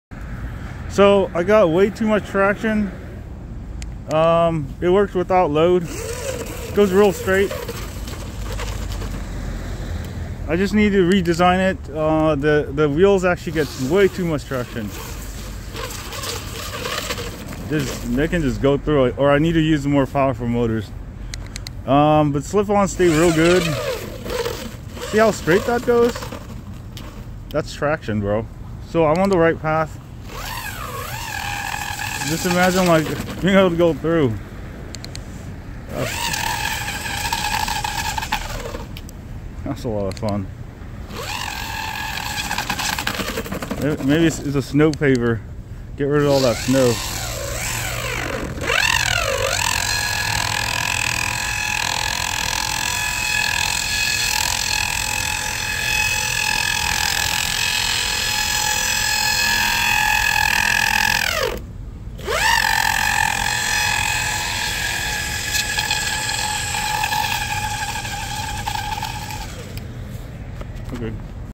Snow Slip On Design has Too Much Traction!
It turns out my new snow slip on design actually has too much traction, will try it on more powerful motors with higher gearing.  In the meanwhile I am designing some new Slip ons with less traction for lower powered motors like this one.